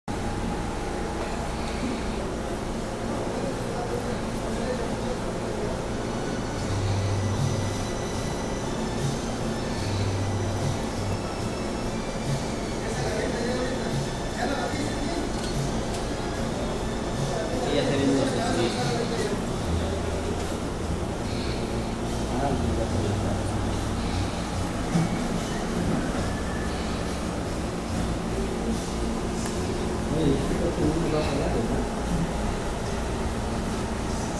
Decoracion de paleteria Michoacana
Ejemplo de uan paleteria que decoramos en Rockford IL